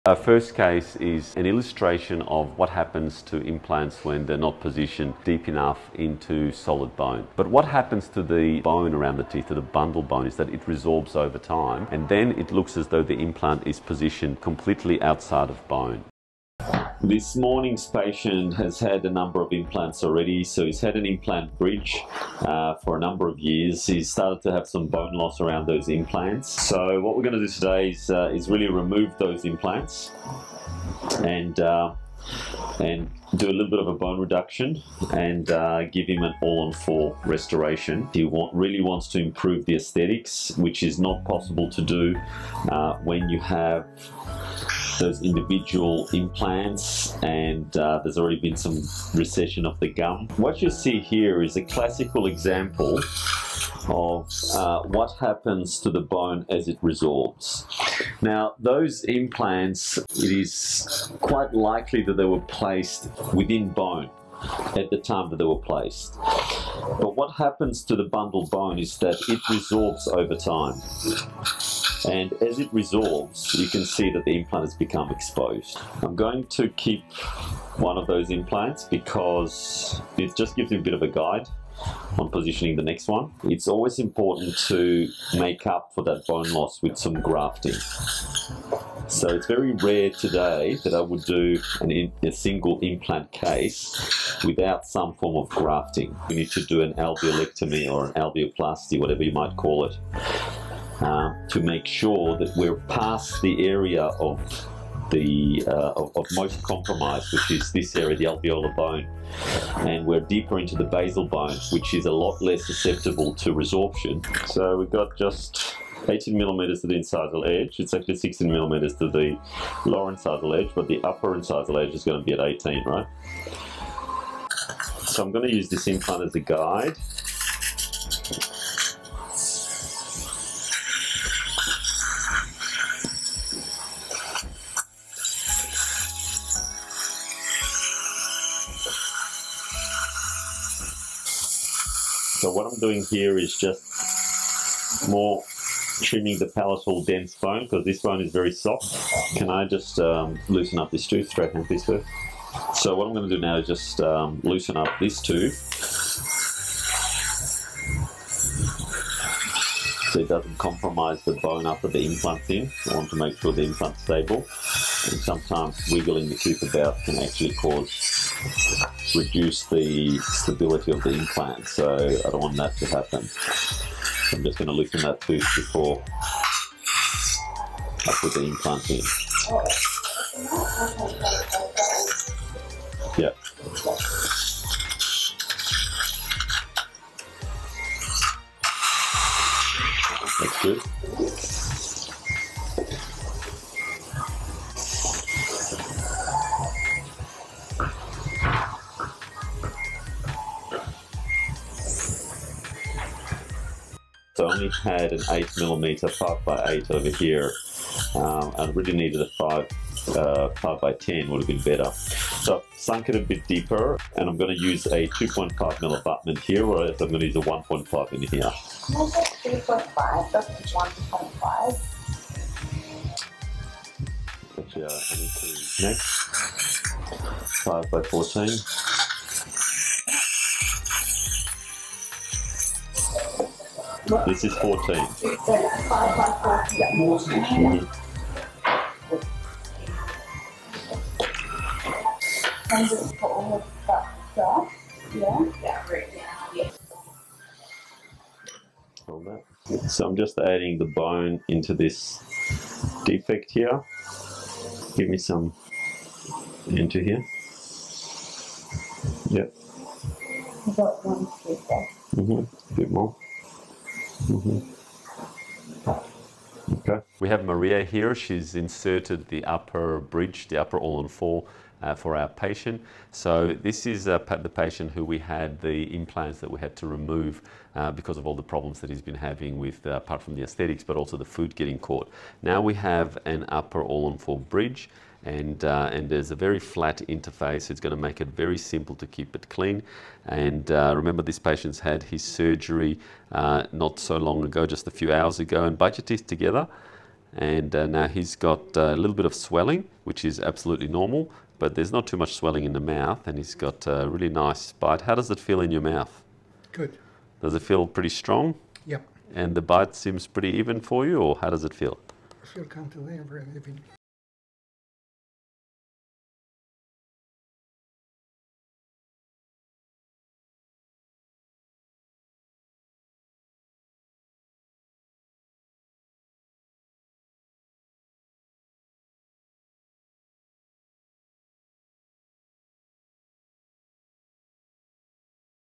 Dr. Alex Fibishenko - Pikos Symposium 2023 - Failed Implant Removal with Full Arch Restoration
Dr. Alex Fibishenko - Pikos Symposium Lecture Preview - Failed Implant Removal with Full Arch Restoration. A glimpse into his greatly anticipated presentation, Full Arch Compendium: From Complications to Success in Total Immediate Implant-based Rehabilitation.

Pikos Symposium 2023 is only 8 weeks away! Seats are selling out fast.